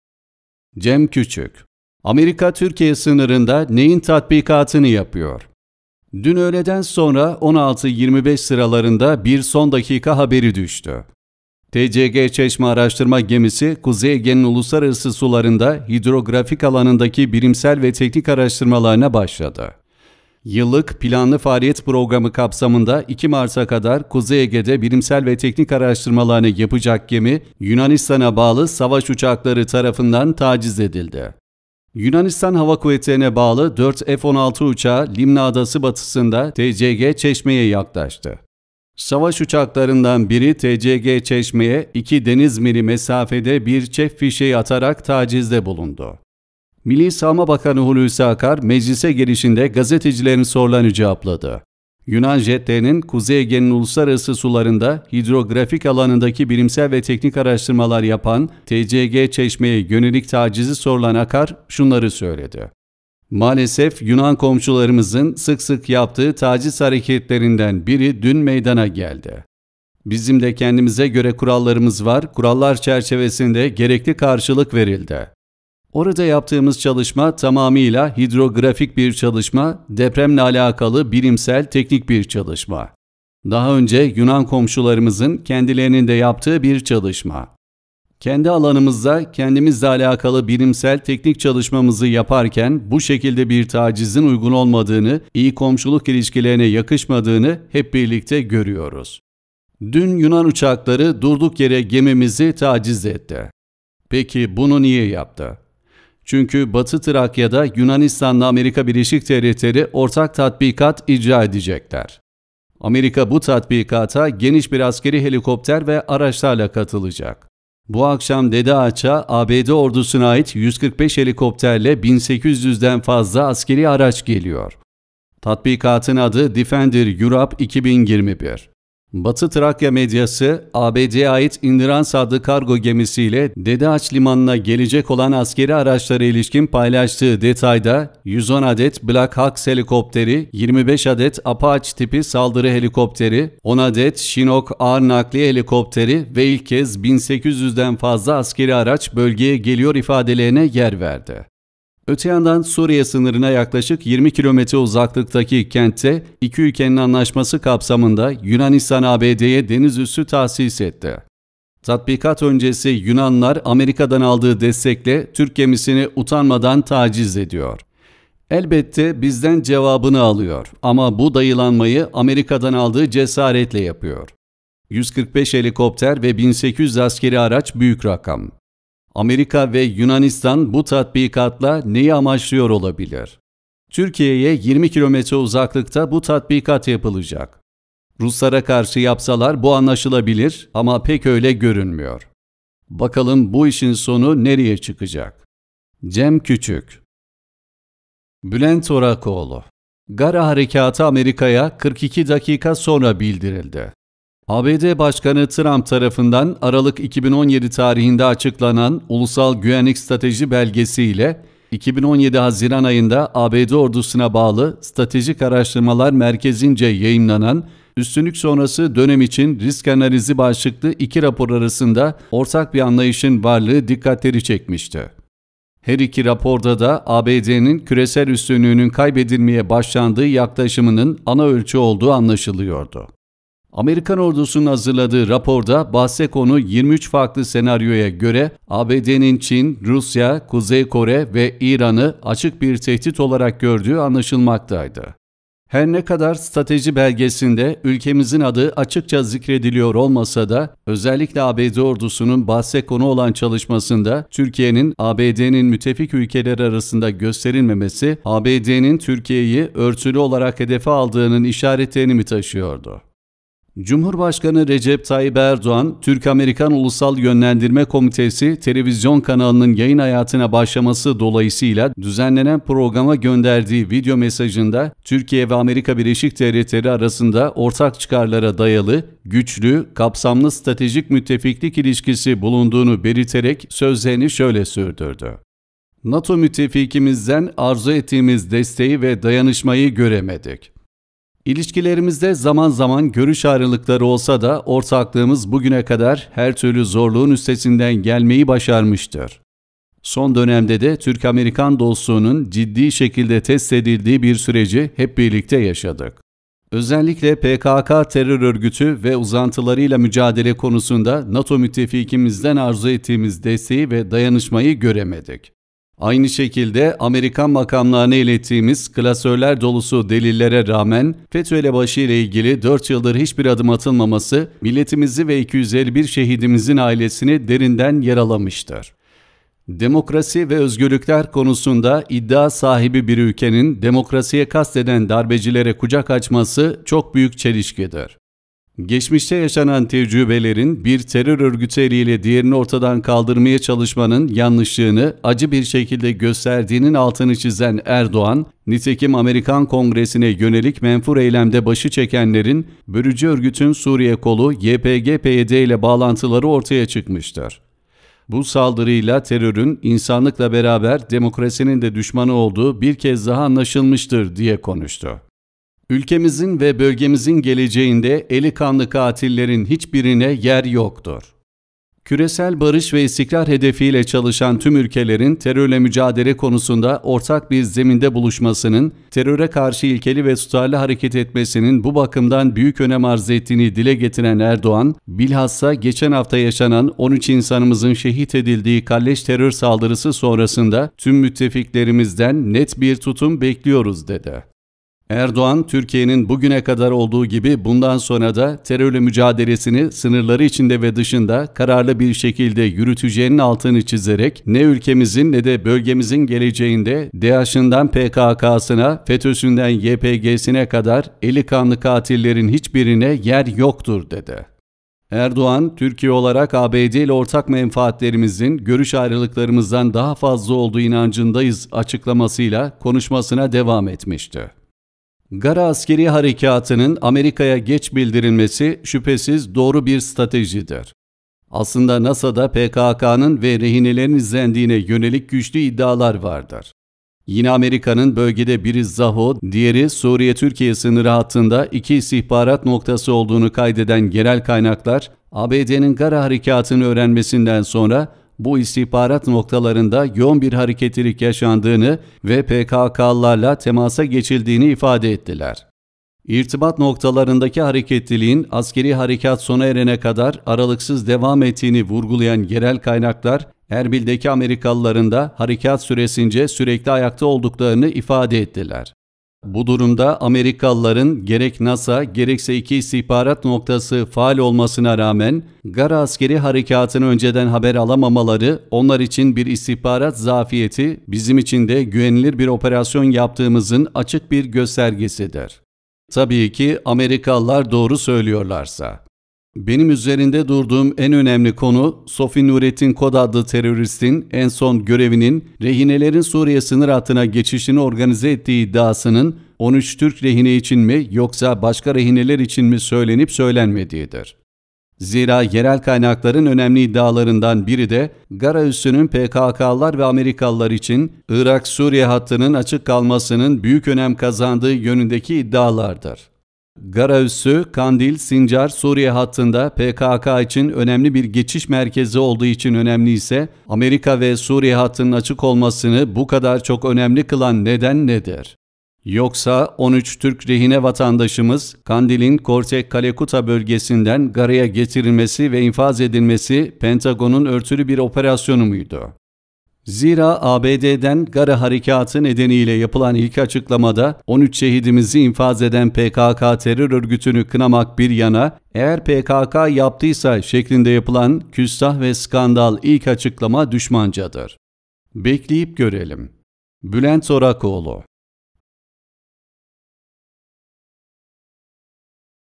BURNUMUZUN DİBİNDE NEYİN TATBİKATINI YAPIYORLAR?.. CEM KÜÇÜK ve BÜLENT ORAKOĞLU YAZDI!
Tatbikat öncesi Yunanlılar Amerika’dan aldığı destekle Türk gemisini utanmadan taciz ediyor. Elbette bizden cevabını alıyor ama bu dayılanmayı Amerika’dan aldığı cesaretle yapıyor. 145 helikopter ve 1800 askerî araç büyük rakam. Amerika ve Yunanistan bu tatbikatla neyi amaçlıyor olabilir?..

Türkiye’ye 20 km uzaklıkta bu tatbikat yapılacak. Ruslara karşı yapsalar bu anlaşılabilir ama pek öyle görünmüyor. Bakalım bu işin sonu nereye çıkacak?..

Gara harekatı Amerika’ya 42 dakika sonra bildirildi!!!
Amerikalıların gerek NASA gerekse iki istihbarat noktası faal olmasına rağmen Gara askeri harekatını önceden haber alamamaları onlar için bir istihbarat zafiyeti bizim için de güvenilir bir operasyon yaptığımızın açık bir göstergesidir...